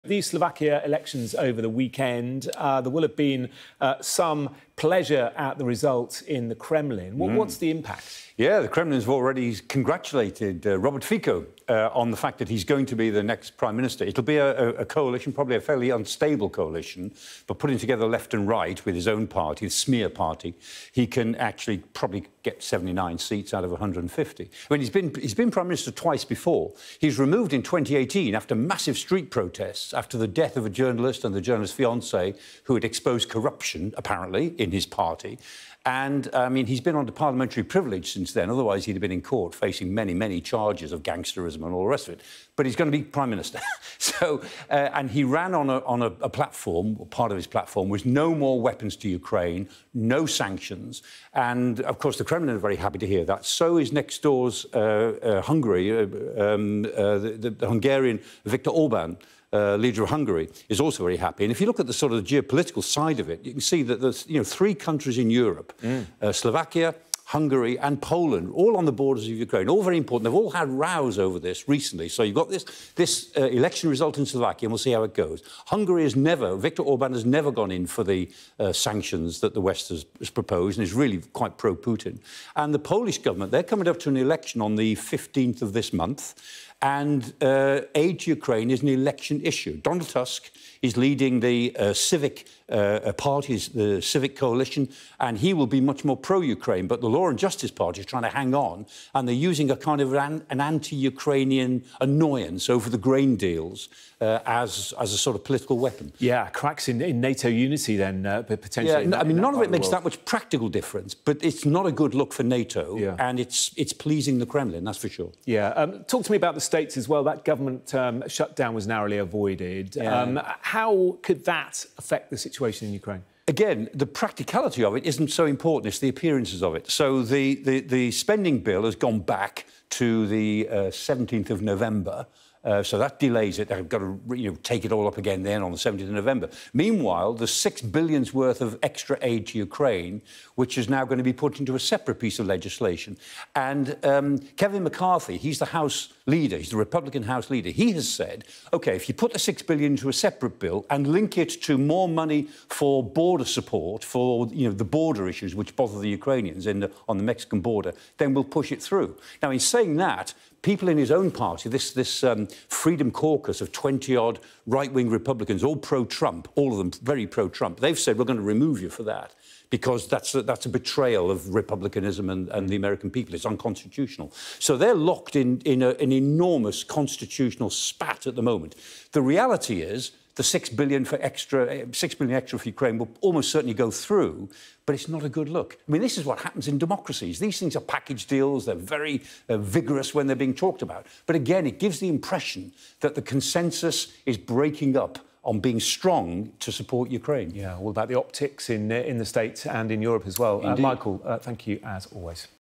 Ukraine War: Will Slovakian elections result break NATO consensus on aid to Kyiv?
Scepticism is suddenly developing in Europe and America about aid for Ukraine.

Professor Michael Clarke stressed the new Slovakian prime minister will likely create an unstable coalition that opposes arms for Ukraine and sanctions for Russia.